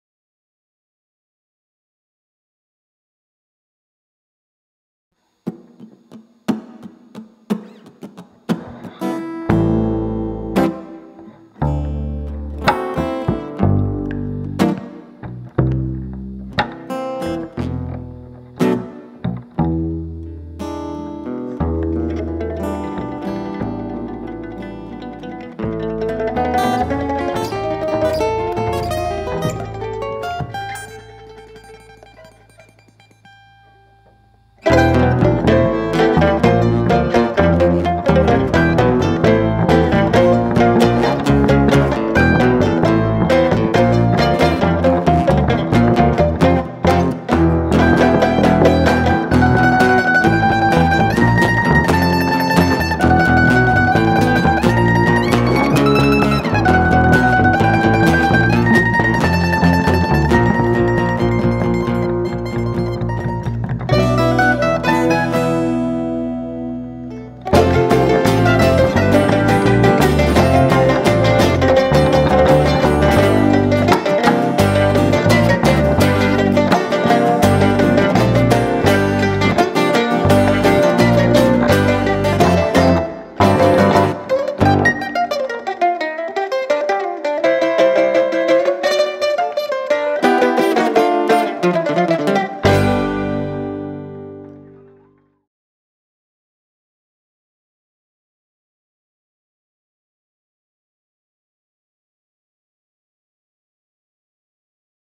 RUSTICIALLEGRI Live at RLT Unplugged 2014
RLT Unplugged 2014 è giunto al penultimo appuntamento. Stavolta è stato il turno dei Rustici Allegri, poliedrico trio romano che ha condito la serata con un po' di bluegrass. Un bel live all'insegna della buona musica e della simpatia.

video a cura di Vibes Art
audio a cura di BackBeat Studio